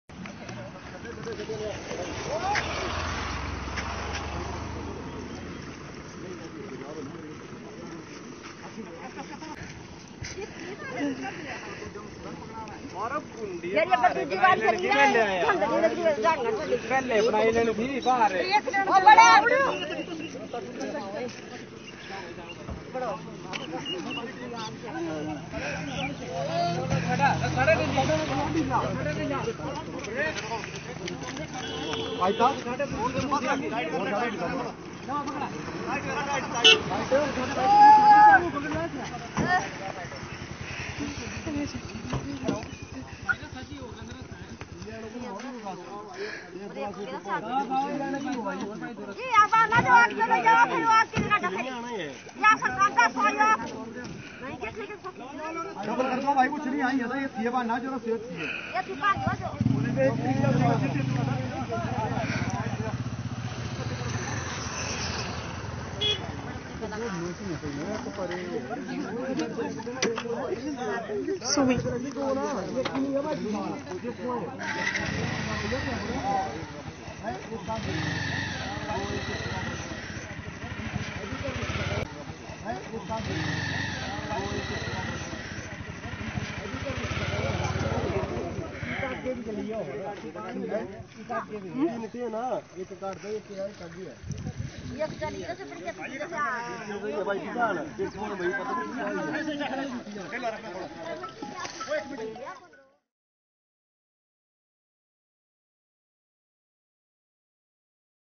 Heroic locals risk their lives to save a bull from drowning | SWNS
This is the moment a group of locals risked their lives to save a bull from drowning in a river after it slipped and fell into the fast flowing water. The video shows brave people using a rope to tie it around the bovine's horns in an effort to pull it out. While a man is tying the rope, another man is forming a chain by holding on to a different rope as they continue to rescue the animal.